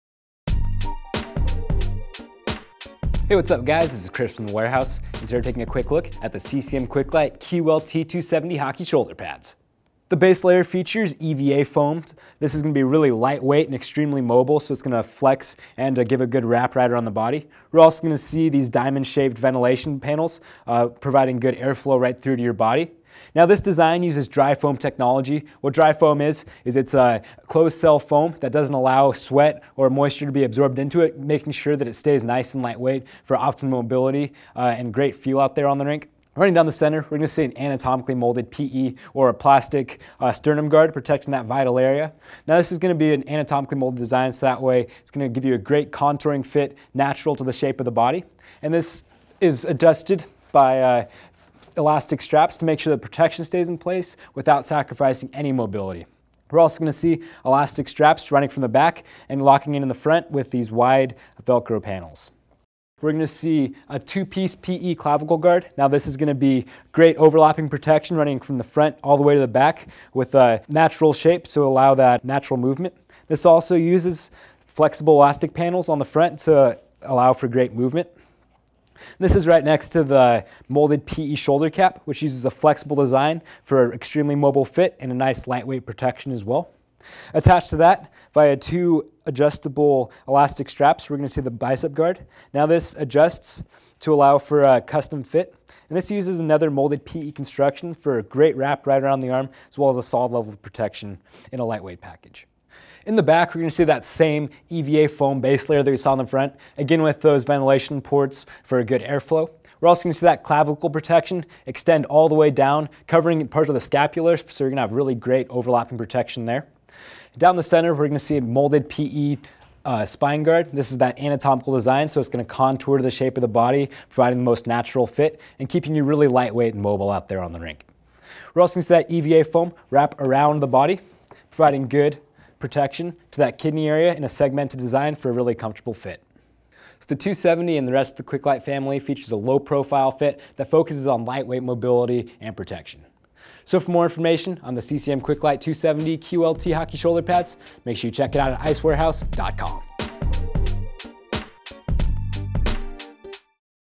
CCM QuickLite QLT 270 Hockey Shoulder Pads Review
IW Hockey reviews the CCM QLT 270 shoulder pads available

The CCM QuickLite QLT 270 Hockey Shoulder Pads uses an extremely lightweight EVA foam base that perfectly contours around the body for incredible mobility. Even with its lightweight fit, the Quicklite 270 provides comprehensive overlapping coverage to maintain mobility without sacrificing protection. This is especially noticeable in the clavicle, shoulder, and bicep protection...